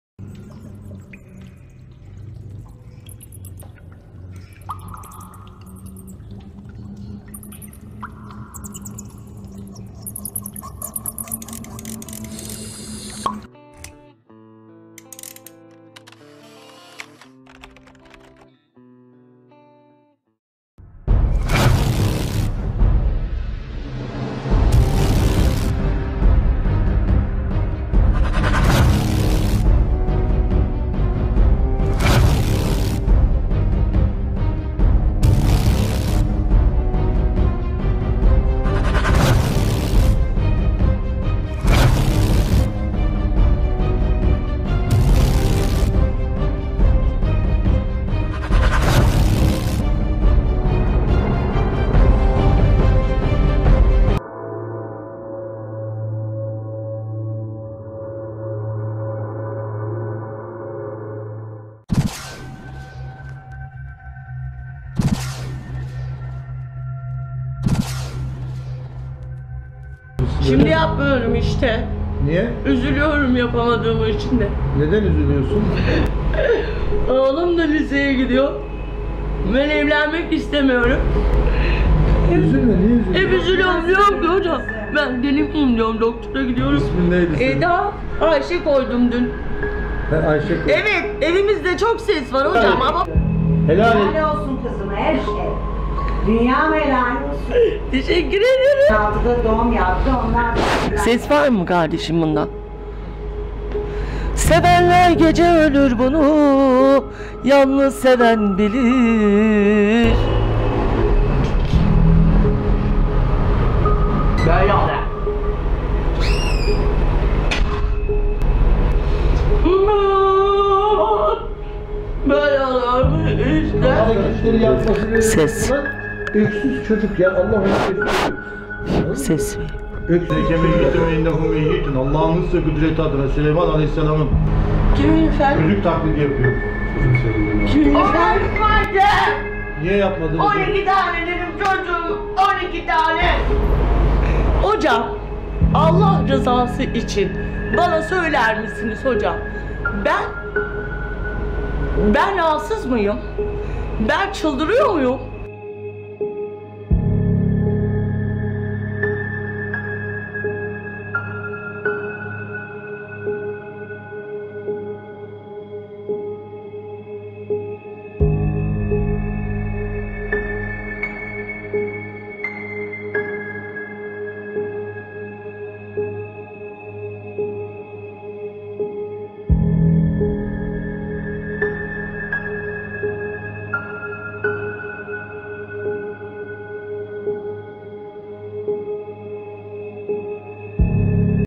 PARANORMAL OFİS 278 "Tevbe-i Sayik" ŞAŞIRTICI SEANS..! ÇOCUĞA DÖNÜŞEN KADIN (fragman) 5k55
SPİRİTUALİST ARAŞTIRMACI YAZAR EROL TANGUT
TEVBE-i SAYİK ve ŞEYTAN HARİTASI KİTAPLARI
 Maddi ve Manevi  tüm dertlerinizin devası
Tevbe Ve Dua mucizeleri
35 Yaşındaki annenin 8 yaşındaki çocuğa nasıl dönüştüğünü canlı seansta izleyeceksiniz.
SAYİK EROL TANGUT
ULUS/ANKARA/TÜRKİYE